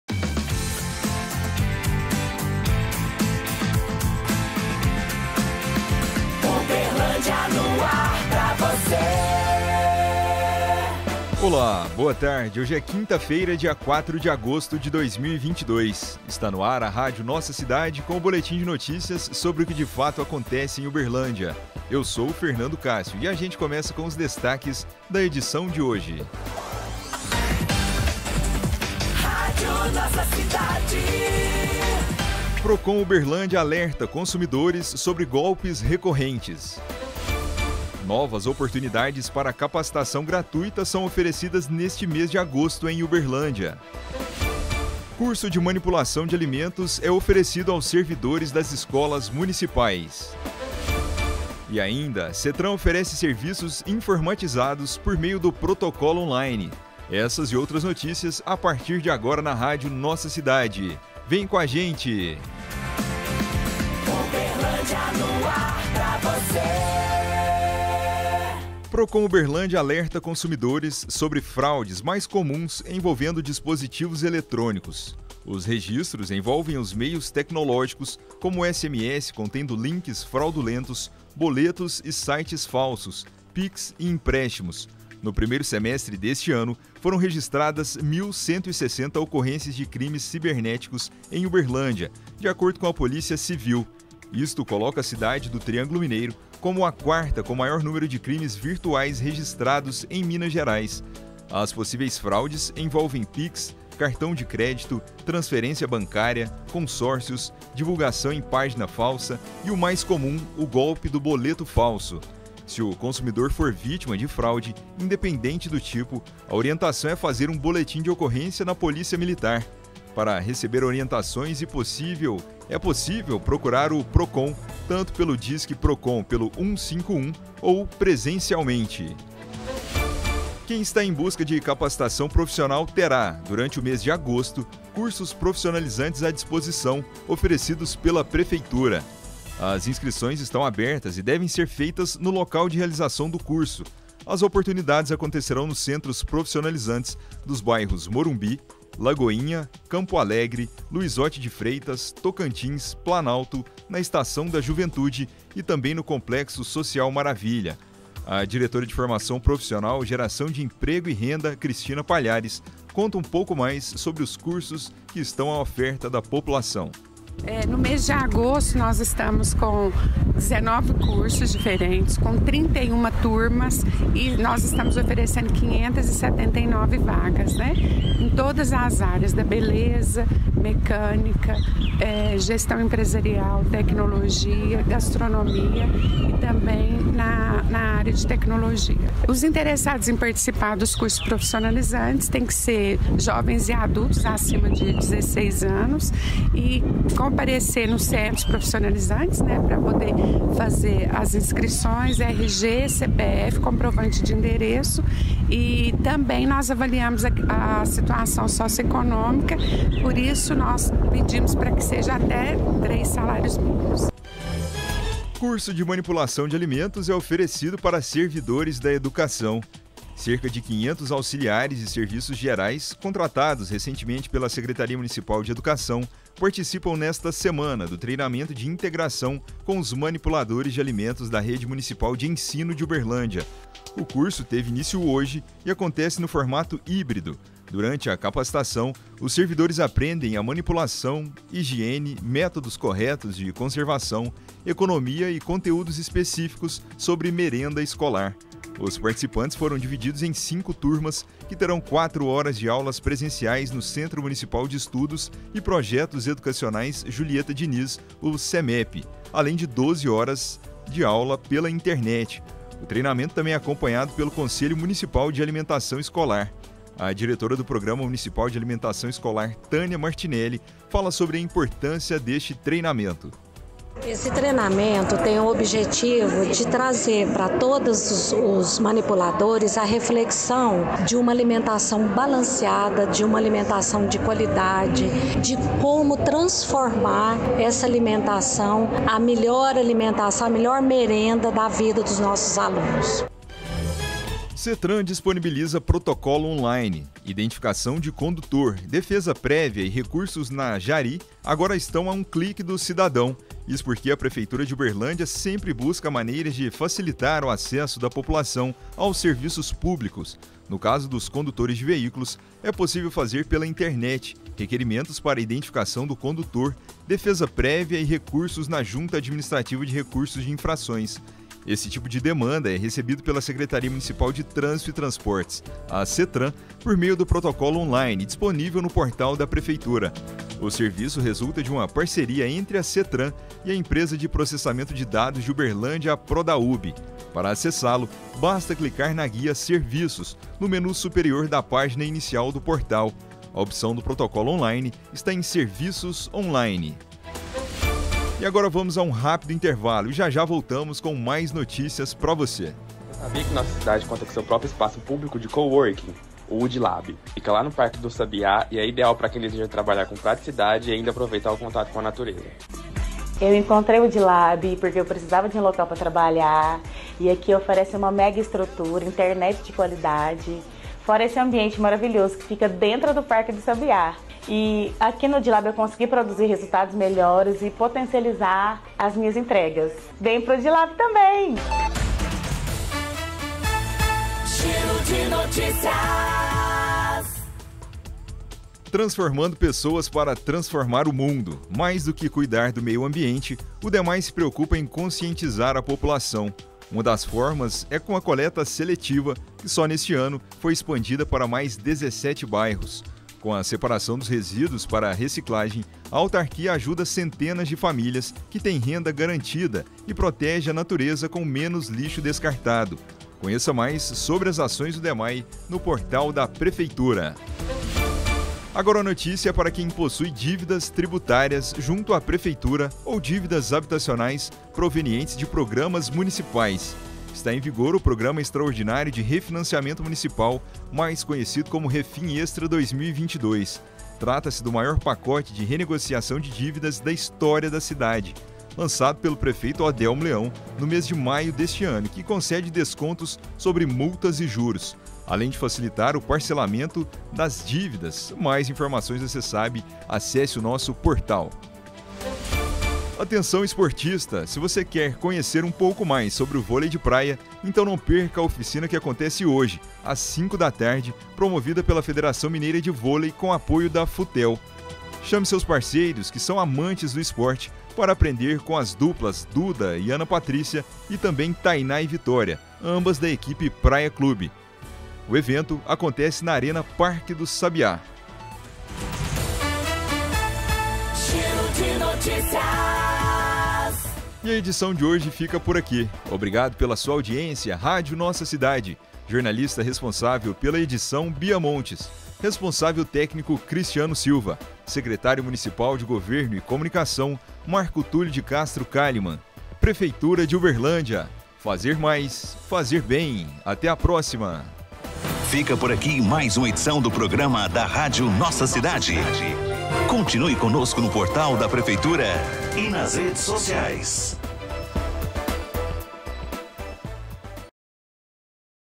#765: Procon Uberlândia alerta consumidores sobre golpes recorrentes
Novas oportunidades para capacitação gratuita são oferecidas neste mês de agosto em Uberlândia, Curso de manipulação de alimentos é oferecido aos servidores das escolas municipais, SETTRAN oferece serviços informatizados por meio do protocolo online